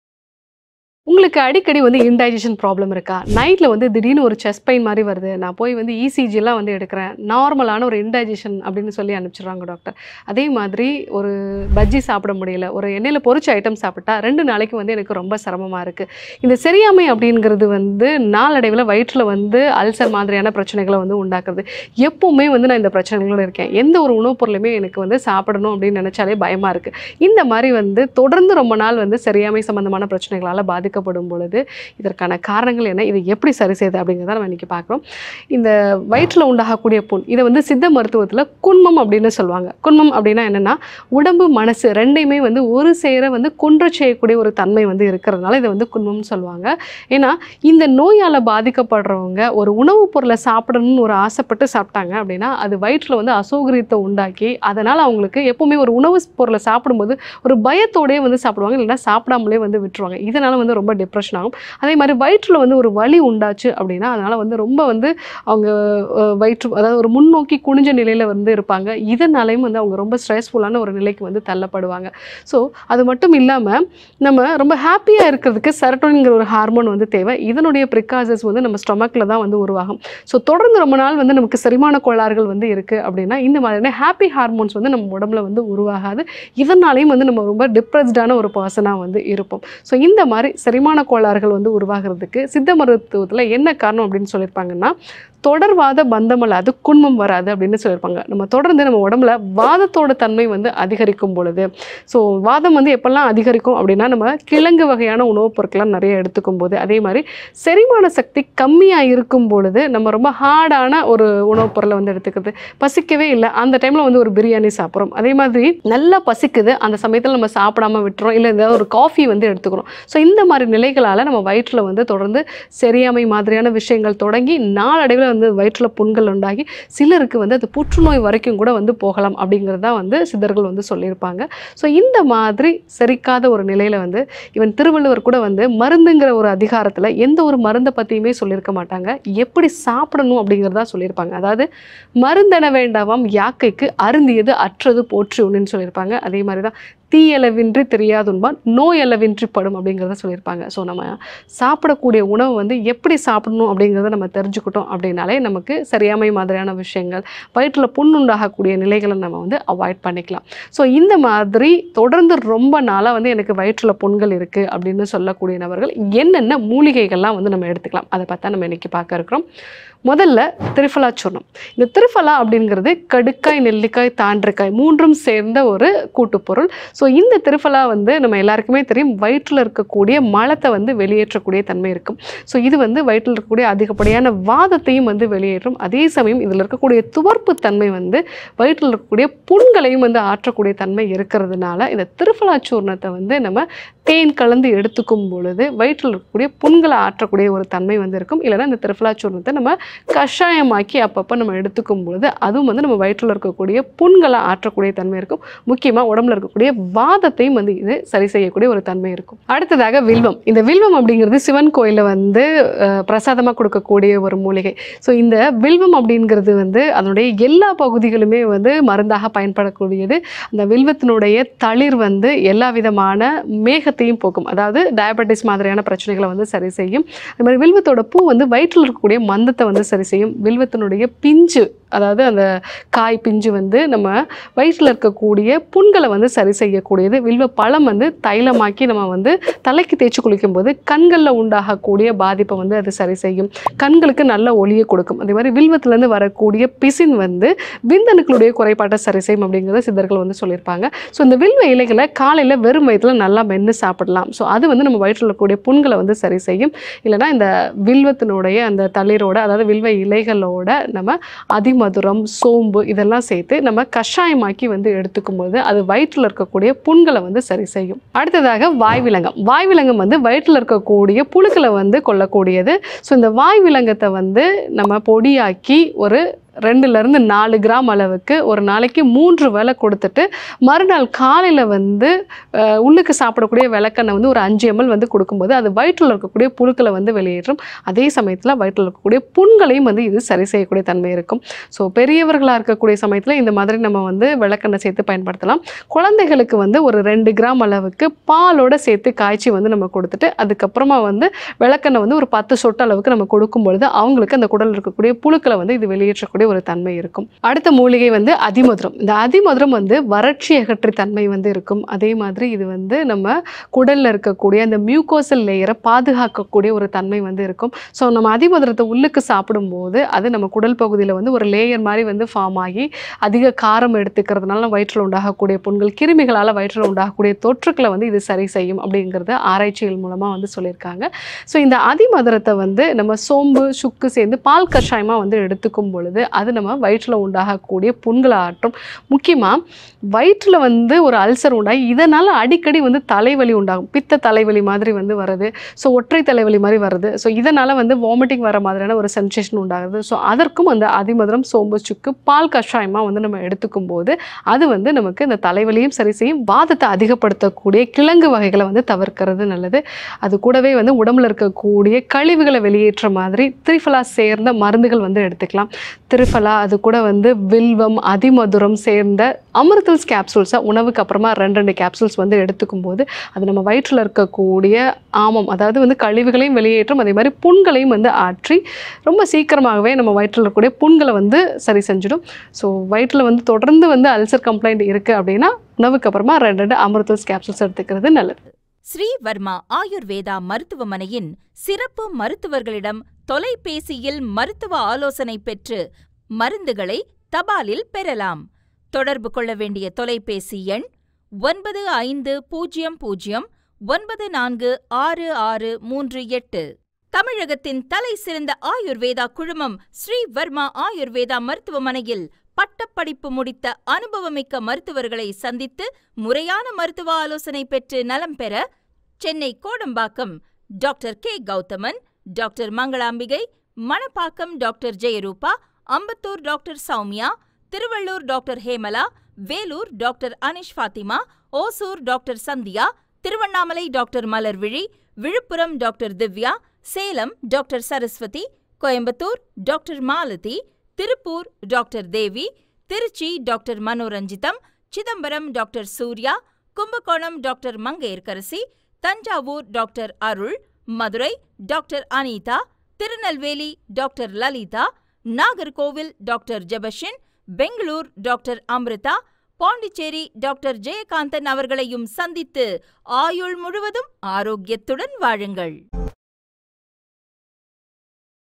Banish digestive woes effortlessly ! செரிமான பிரச்சனைகள் எளிதில் நீங்க வேண்டுமா ?
Banish digestive woes effortlessly ! Simple solutions for a happy gut | செரிமான பிரச்சனைகள் எளிதில் நீங்க வேண்டுமா ?

இன்றைய வாழ்க்கை சூழலில் பலரும் வயிறு சம்மந்தப்பட்ட பிரச்சனைகளால் அவதிப்படுகிறார்கள்.  வயிற்று புண் இதை குன்மம் என்றும்,  வயிறு, மனசு இரண்டையும் ஒரு சேர குன்ற செய்வது இதனால் வயிற்று வலி மற்றும் அசௌகரியம் ஏற்பட்டு அவதிப்படுவார்கள்.  செரிமான கோளாறுகள் உள்ளவர்கள் கடினமான உணவுகளை தவிர்க்க வேண்டும். பசி ஏற்பட்டும் உணவை தாமதமாக உண்பது தீராத விளைவுகளை ஏற்படுத்தும்.

திரிபலா சூரணம் இதில் உள்ள துவர்ப்பு தன்மை வயிற்று புண்களை ஆற்றும்.வில்வம் இதில் உள்ள அனைத்து பாகங்களும் மருந்தாக பயன் தரும், சர்க்கரை நோயை குணப்படுத்திடும். வாய்விளங்கம் குடல் புழுக்களை  அழிக்கும். அதிமதுரம் இது குடலில் உள்ள மியூகோசல் லேயரை பாதுகாக்கின்றது மேலும் சோம்பு, சுக்கு சேர்த்து பால் கஷாயமாக குடித்து வர வயிற்று அல்சர் குணமடையும்,  ஸ்ரீ வர்மாவின் amruthuls கேப்ஸுல்ஸ் உணவுக்கு பின் இரண்டு கேப்ஸுல்ஸ் எடுத்துக்கொள்வதினால் உடல் கழிவை வெளியேற்றி ஆரோக்கியமாக  வாழமுடியும்.

Dr. Jeya Roopa, B.S.M.S, M.D
Medical Director
Shree Varma Ayurveda Hospitals